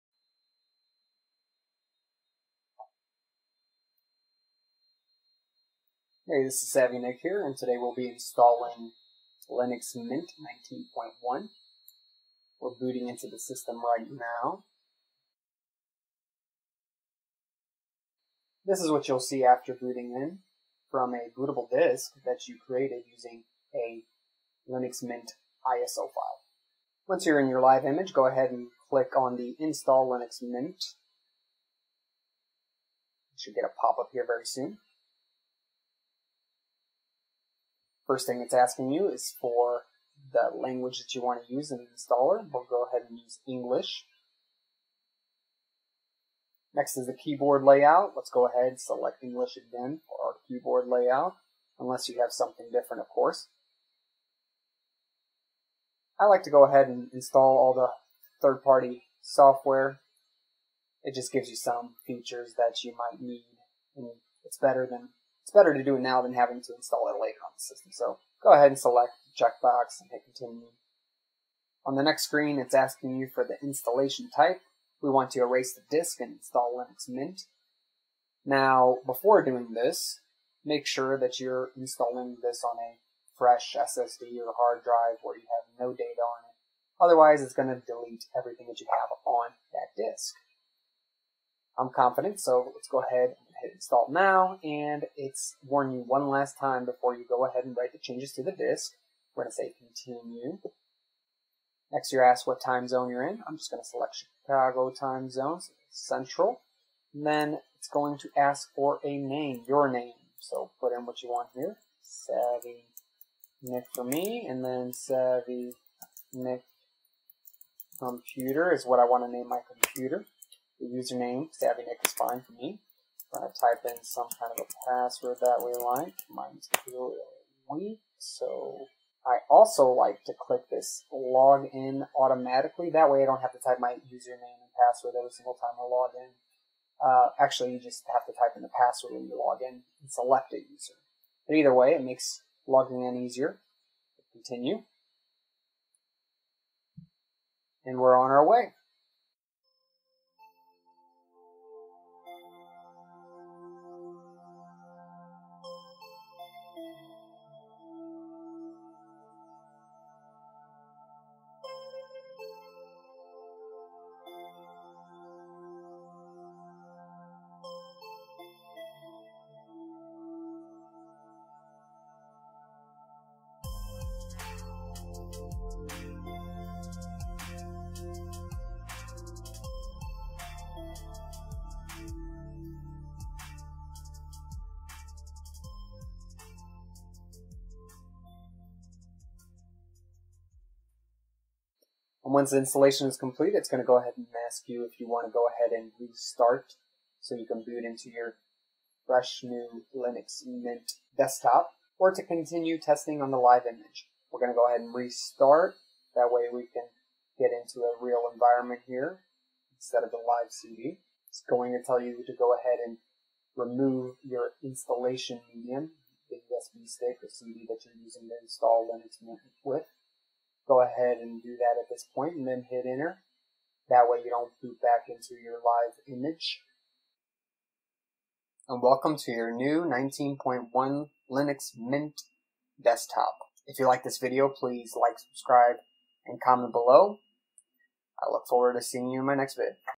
Linux Mint 19 Install Tutorial (Beginners Guide)
A tutorial for how to install Linux Mint 19.1 for beginners.  Easily run through the steps required to successfully install Linux Mint 19.1 on a system of your choice.

This tutorial is on how to install Linux Mint 19.1 on a computer and/or virtual machine.  Easily run through the steps required to successfully install Debian on a system of your choice.